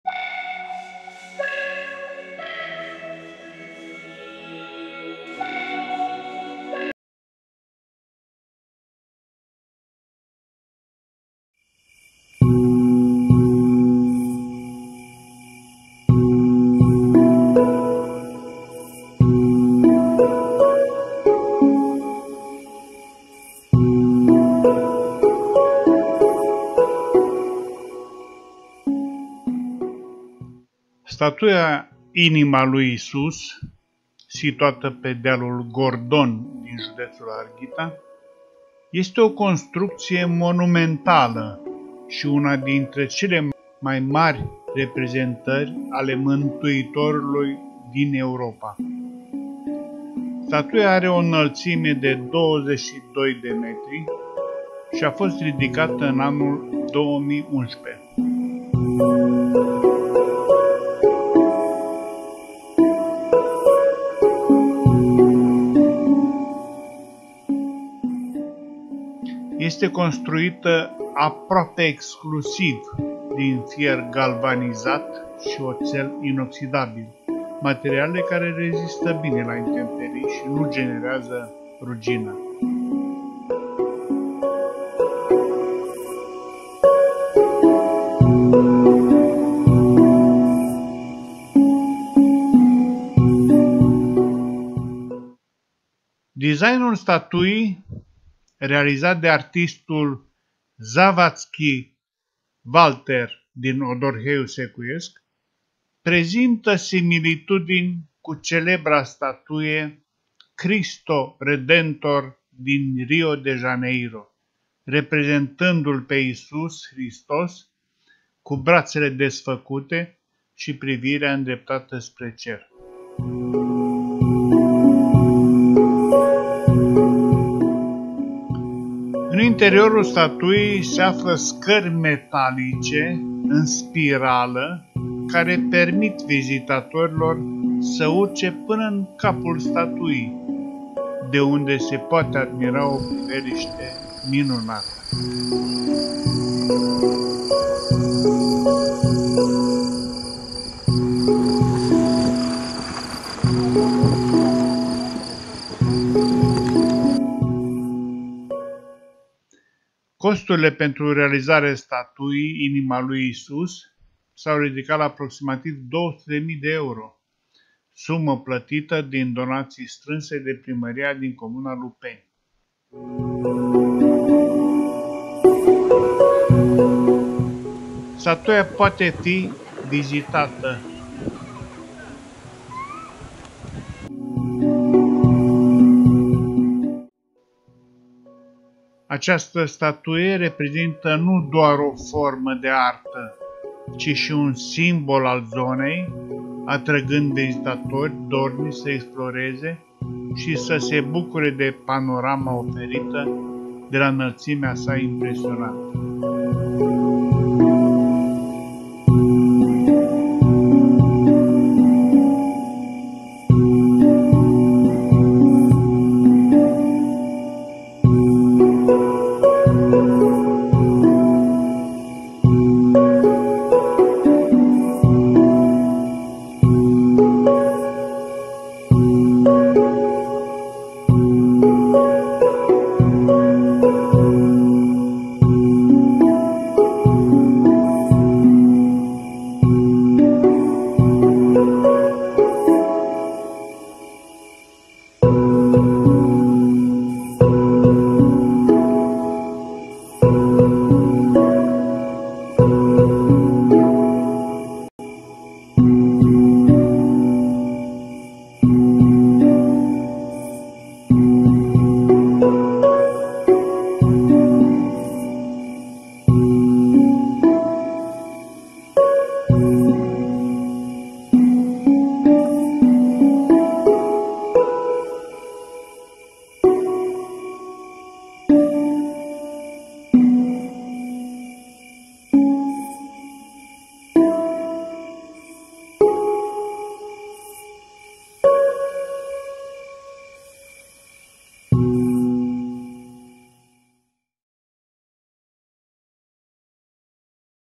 Inima Lui Isus
Cum poți ajunge în locul unde se află „Inima lui Iisus”
Dacă te-ai convins că trebuie să ajungi neapărat să vezi cu ochii tăi statuia „Inima lui Iisus”, ei bine, află că există două variante. Prima ar fi să lași mașina în parcarea de la baza dealului pe care se află impunătorul edificiu. Urcarea este puțin cam abruptă, dar nu durează foarte mult. După 20 de minute de mers prin pădure vei putea admira frumusețea statuii.
A doua variantă prevede de mersul cu mașina. Puți urca cu autoturismul chiar până în spatele statuii, însă trebuie să știi că sunt unele porțiuni cu gropi mari. De asemenea, în zilele ploioase se formează noroi și sunt șanse să rămâi blocat la mijlocul drumului.
Dacă ai noroc și ajungi în zile senine, datorită faptului că statuia este înaltă de 22 de metri, din vârful ei vei putea admira o priveliște spectaculoasă, aproape tot peisajul dintre Odorheiul Secuiesc și Sovata.